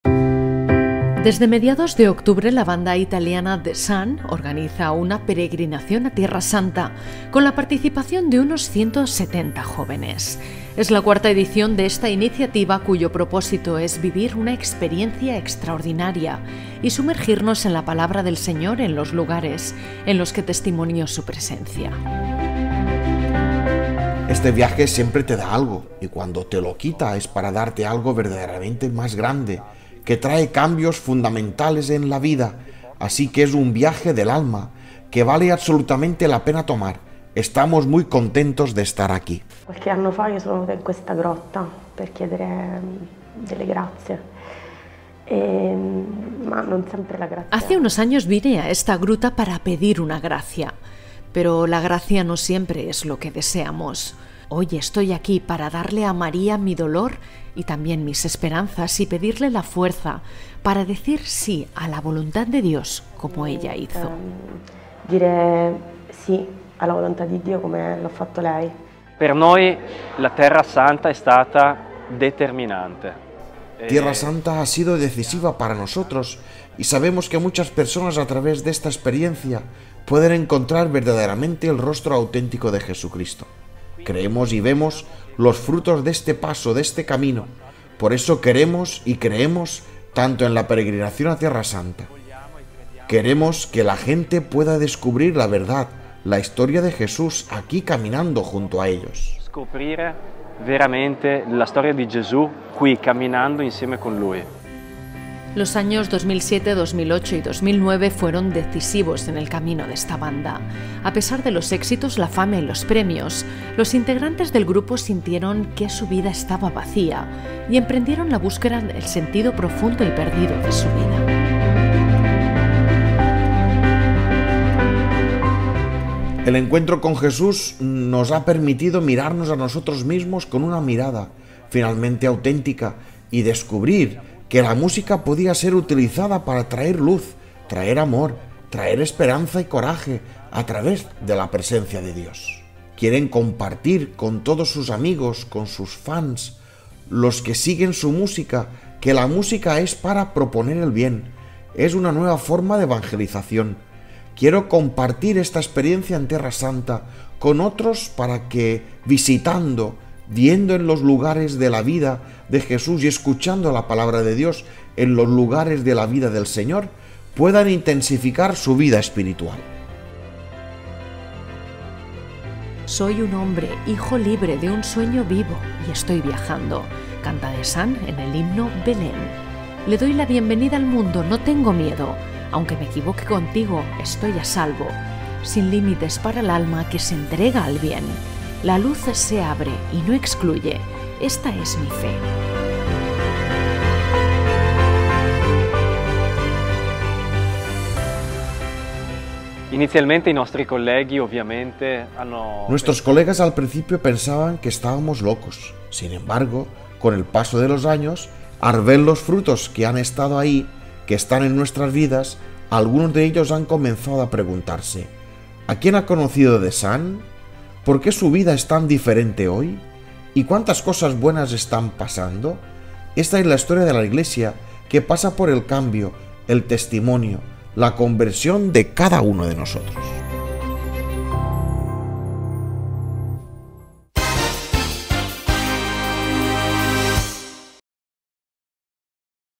Los “The Sun” en Tierra Santa. Con la música al servicio del bien
La banda italiana de música cristiana “The Sun” organizó la cuarta peregrinación a Tierra Santa, con la participación de unos 170 jóvenes.